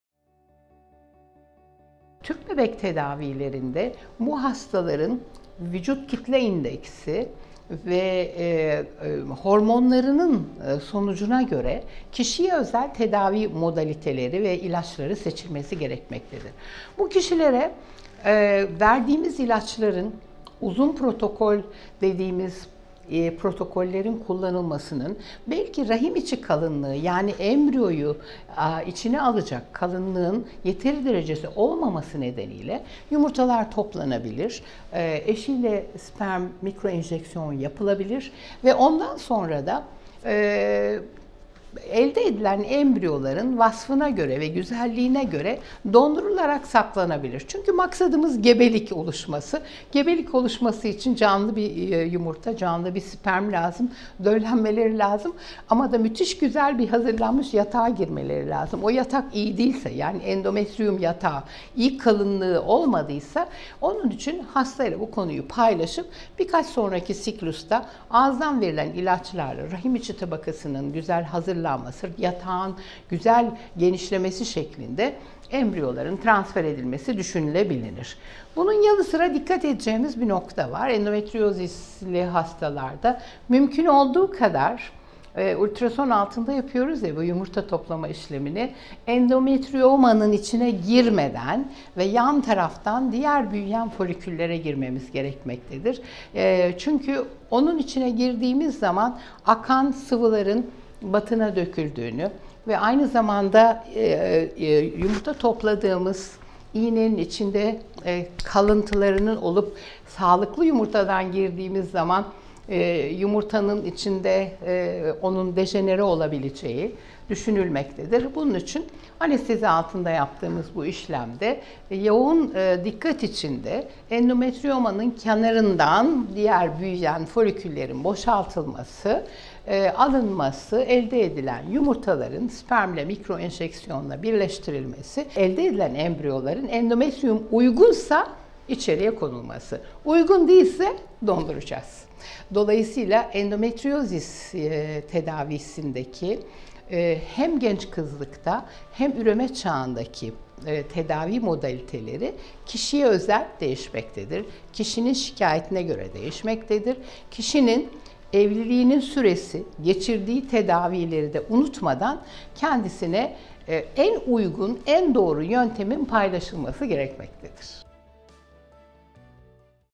Prof. Dr. Oya Gökmen - Tüp bebekte kişiye özel tedavi
Kadın Hastalıkları ve Doğum Tüp Bebek Uzmanı Prof. Dr. Oya Gökmen ''Polikistik Over tedavisi'' hakkında bilgi veriyor.